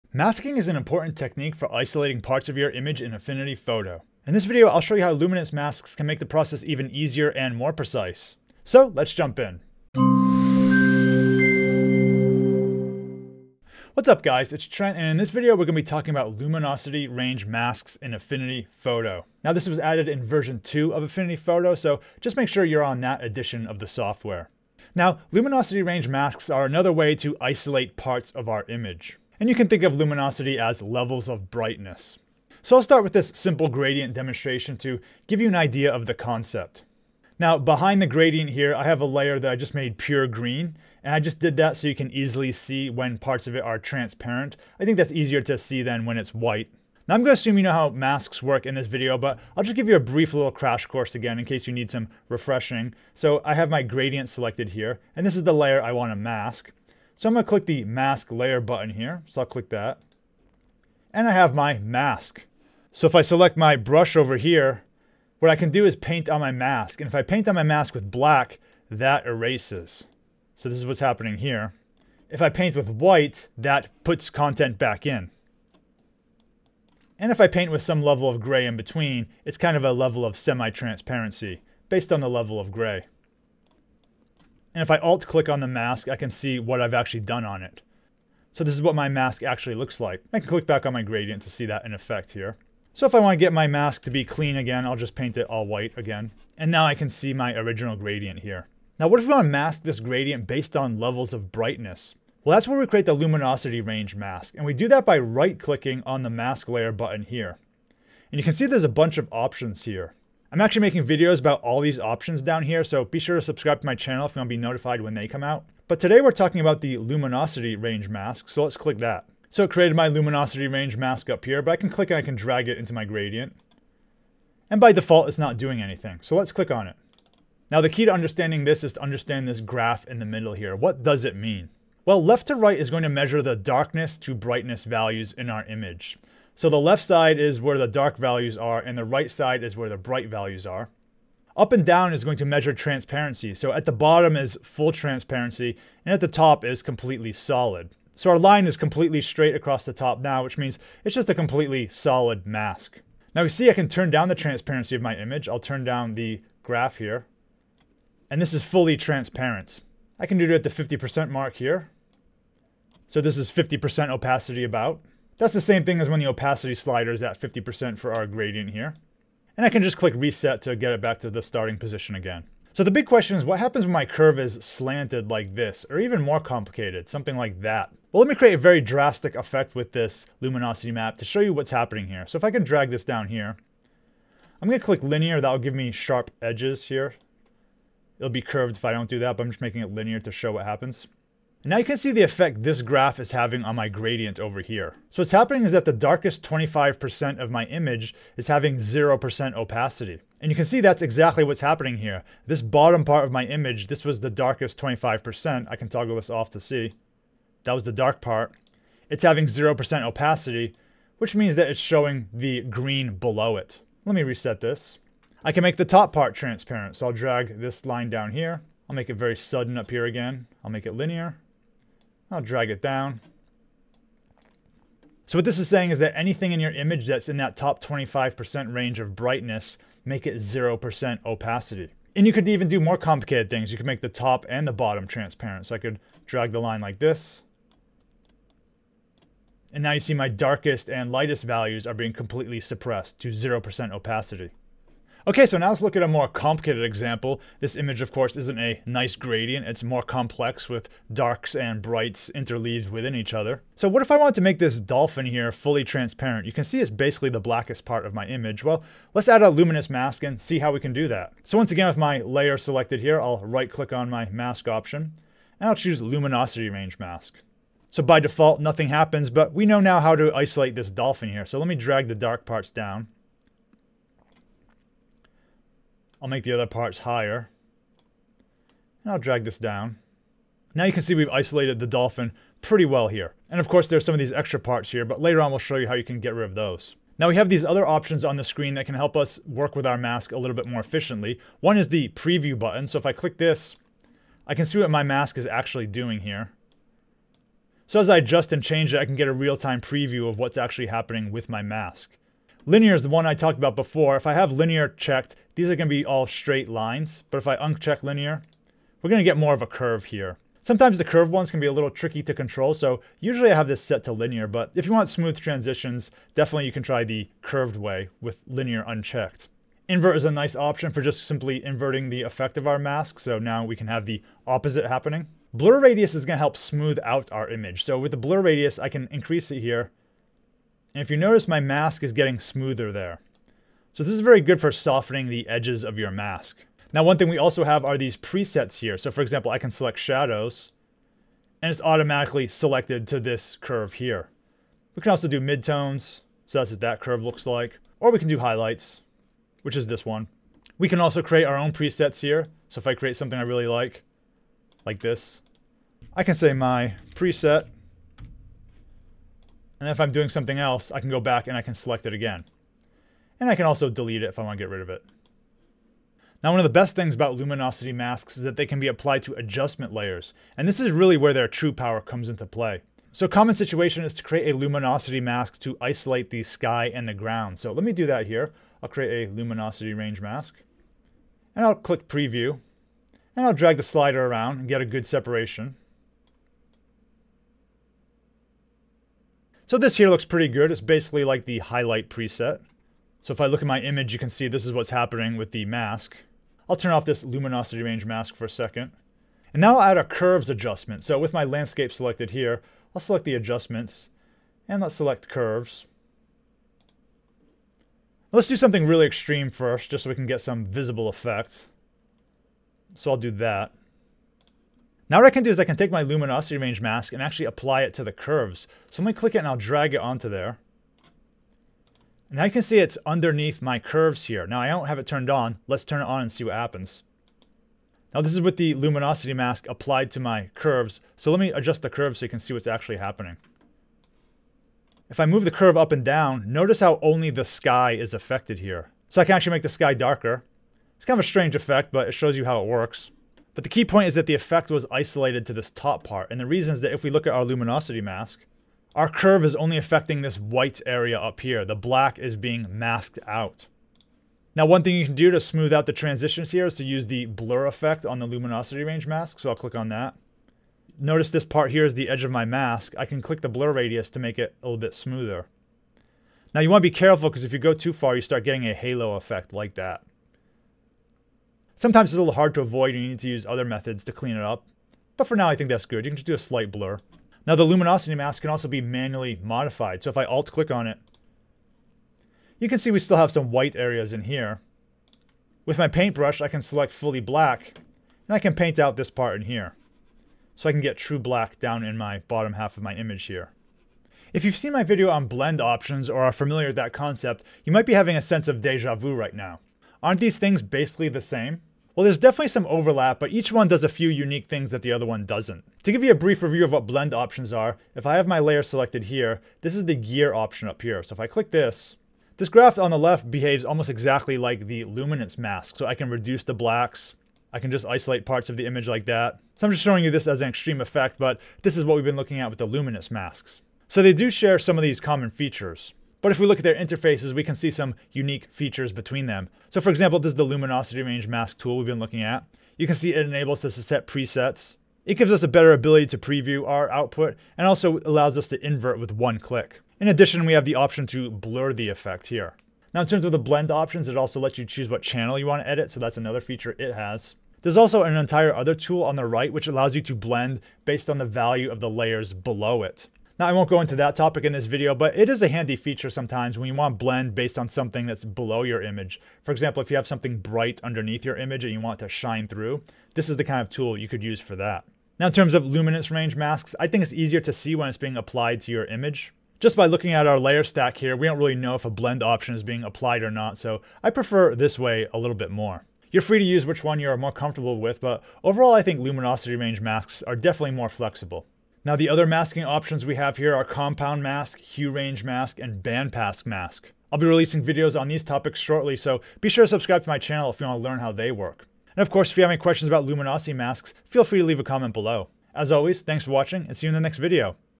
Luminosity Range Mask - Tutorial for Affinity Photo
Luminosity Masks are a way to dynamically create masks based on levels of dark and light. In this video, we’ll look at this feature from the ground up and see how it can be applied to your photographs.

CONTENTS
00:00 Intro
00:46 Mask Review
01:38 Luminosity Range Masks
04:04 Mask Options
06:06 Using With Adjustment Layers
08:13 How Blend Options Compare